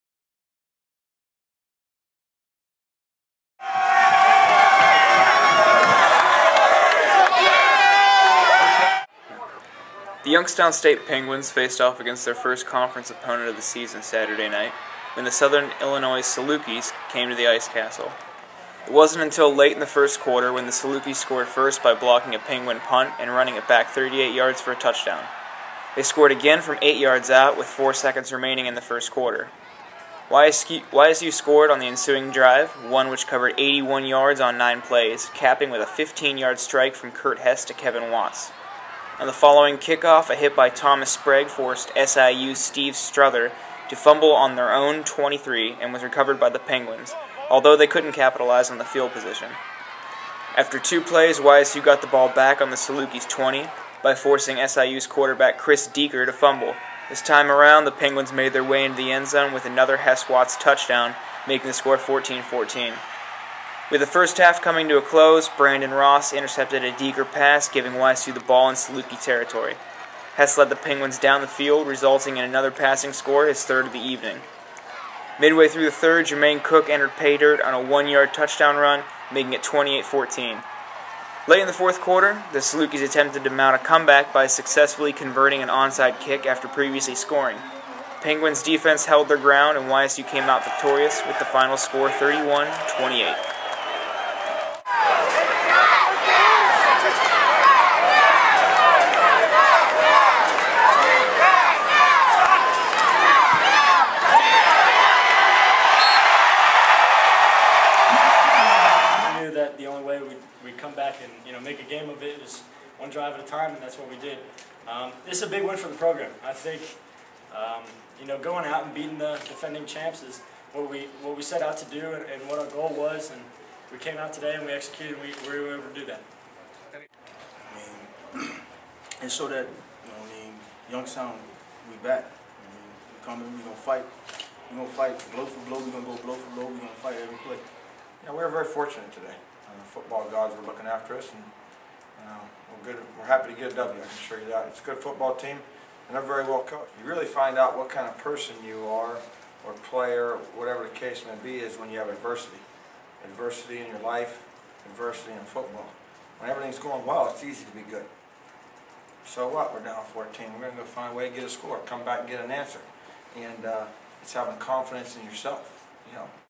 YSU Vs. Southern Illinois Salukis
YSU wins their first conference game of the 2010 season in the Ice Castle.